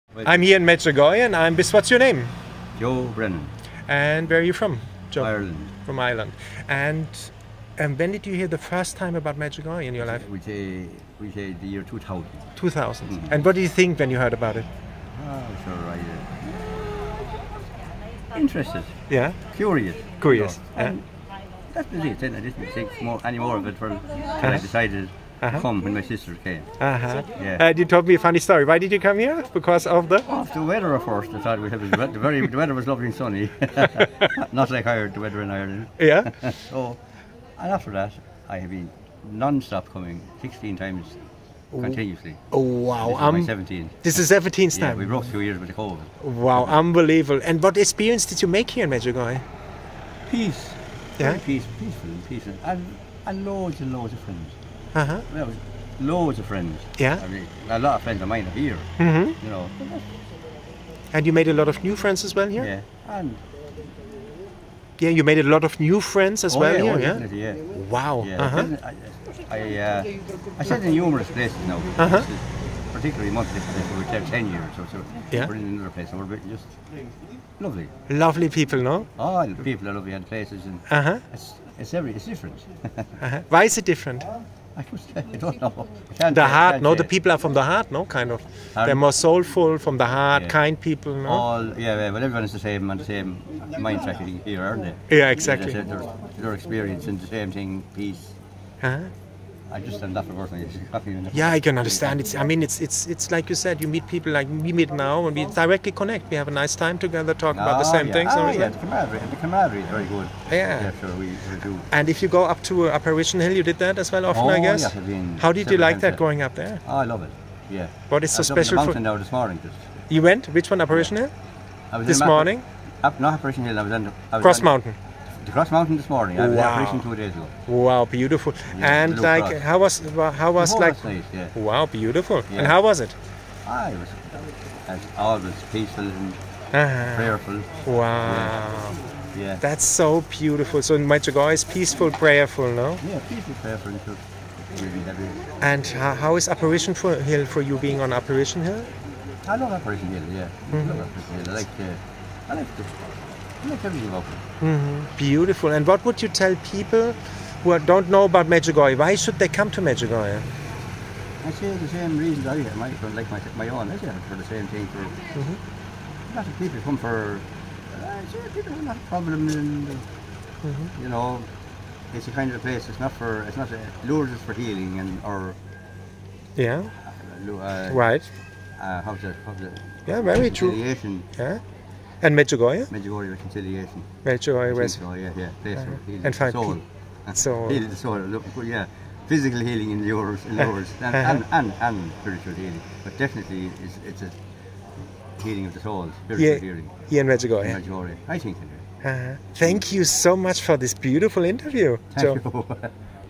Testimony | Joe from Ireland | Medjugorje is a place of spiritual healing
Thomas Huth